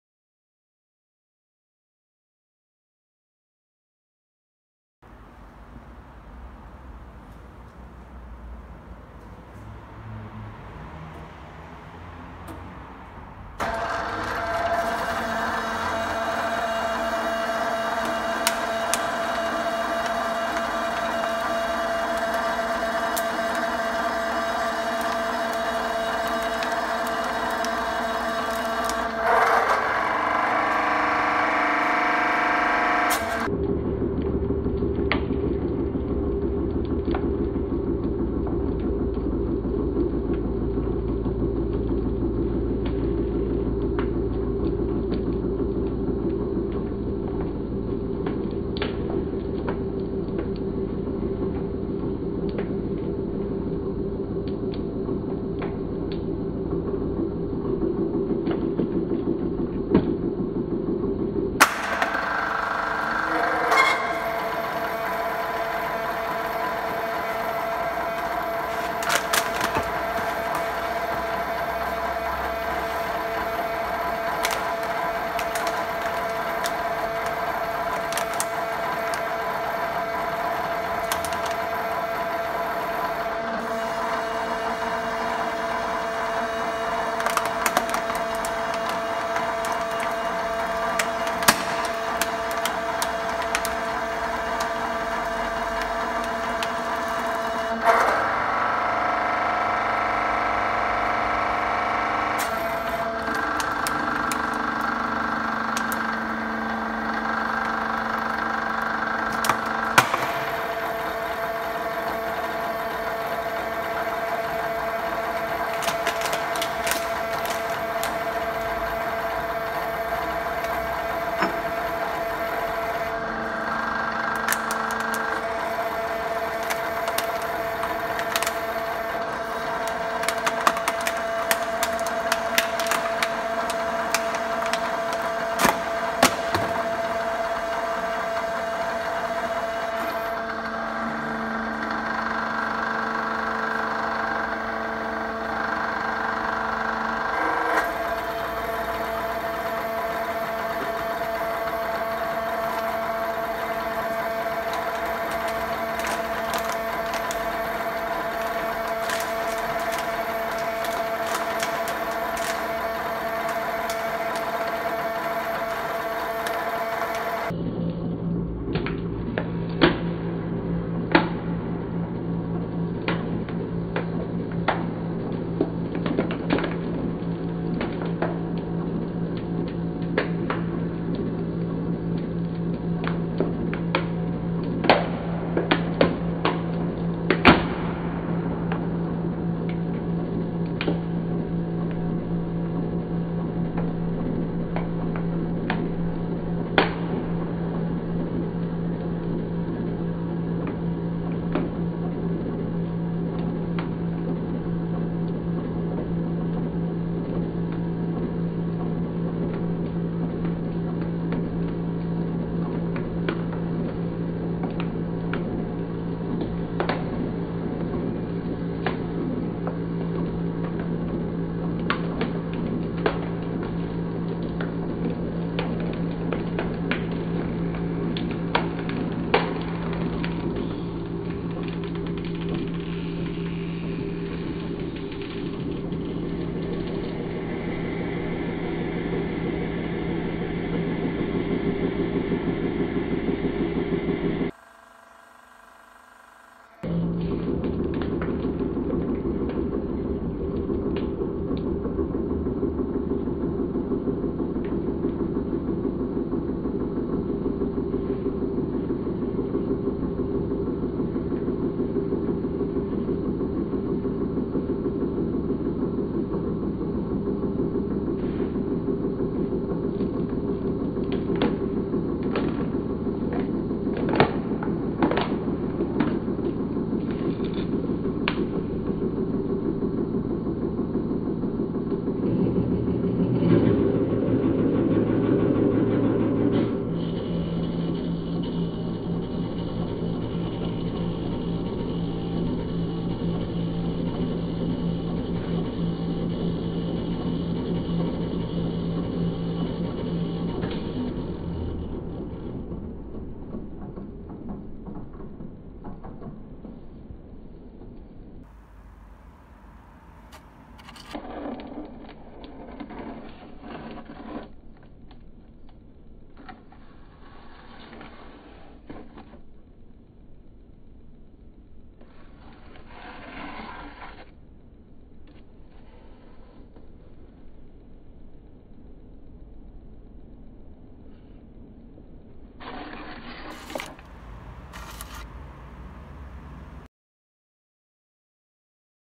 Plastic bottle under press / Bottiglia di Plastica in Pressa
A Plastic bottle can be soo much pain in a hydraulic press. Sit back and enjoy the Suffering 😉